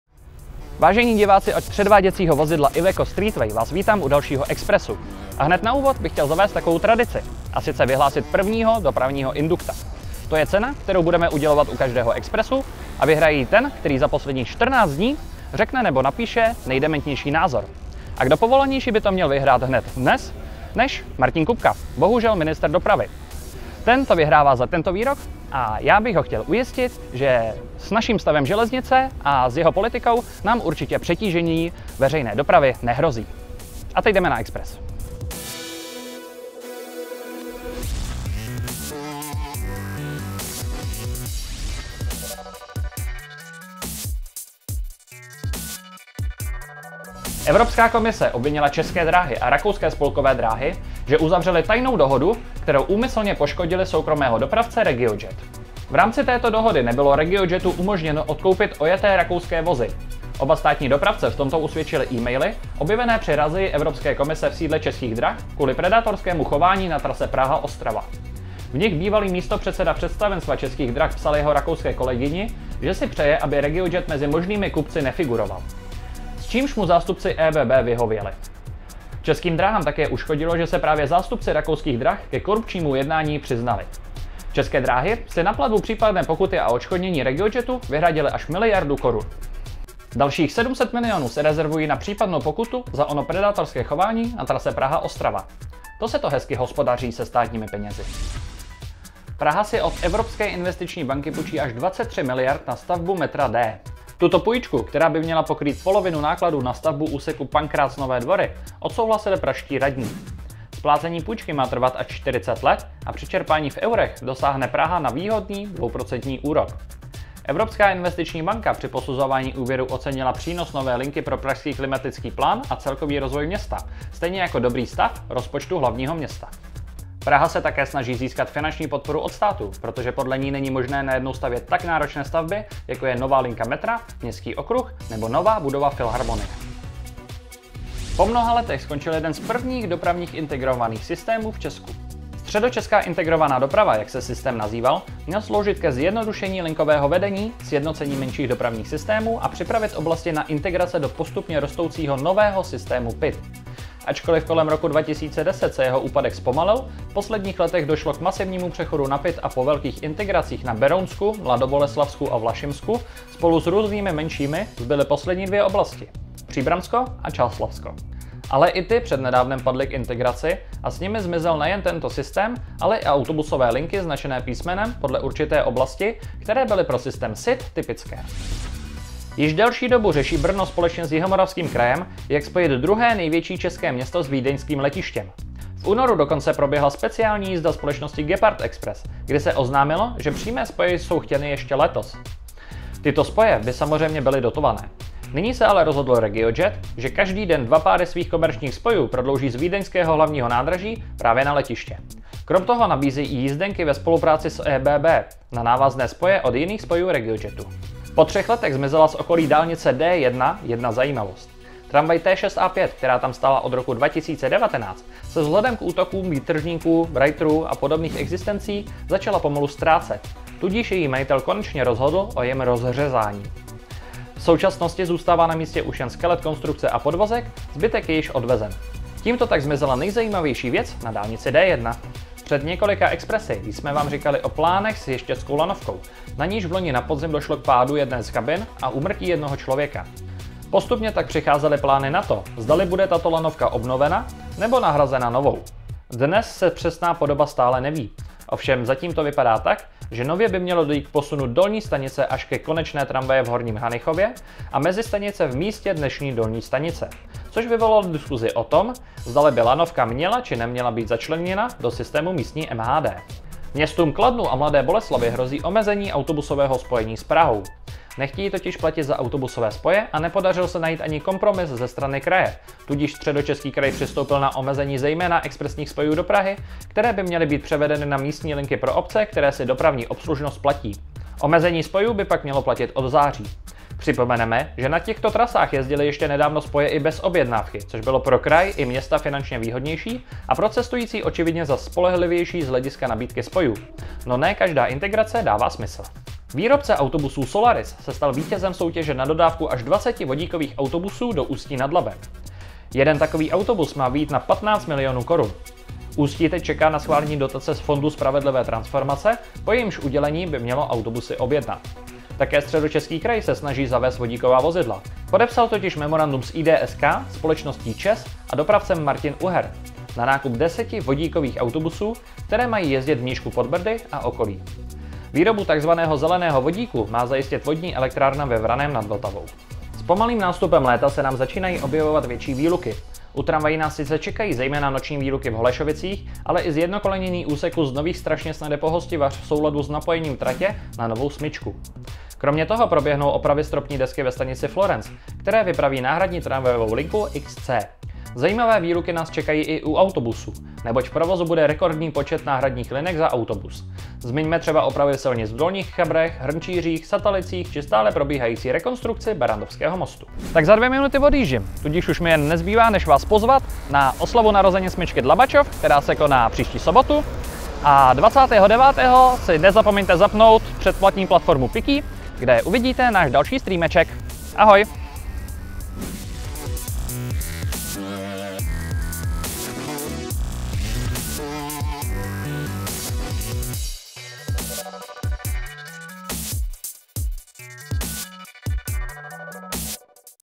METROBUS EXPRES 2022#12: Středočeská integrovaná doprava skončila
Jeden z nejstarších zárodků integrovaných dopravních systémů byl plně pohlcen Pražskou integrovanou dopravou. Historie písmenných autobusových linek ve Středočeském kraji je tak ukončena.

V Expresu #12 uvidíte:
Kartel ČD s ÖBB (seznamzpravy.cz)
Půjčka na metro D (zdopravy.cz)
Konec SID (arriva.cz)
Regiojet na Vídeňské letiště (zdopravy.cz)
Čestlická tramvaj (zdopravy.cz)
Ještědská lanovka (zdopravy.cz)
Kladno vs autobusy (zdopravy.cz)
Vodníci (zdopravy.cz, zdopravy.cz)

Časové značky:
0:00 Úvod
0:49 Kartel ČD a ÖBB
1:39 Půjčka na metro D
2:24 Jeden z prvních IDS končí
3:02 Komerční vlak na Brněnské letiště
3:28 Čestlická tramvaj zmizela
3:51 Ještědská lanovka v MHD?
4:26 Kladno a Boleslav vs autobusy
4:58 Vodníci
5:27 Výluky
6:02 Závěr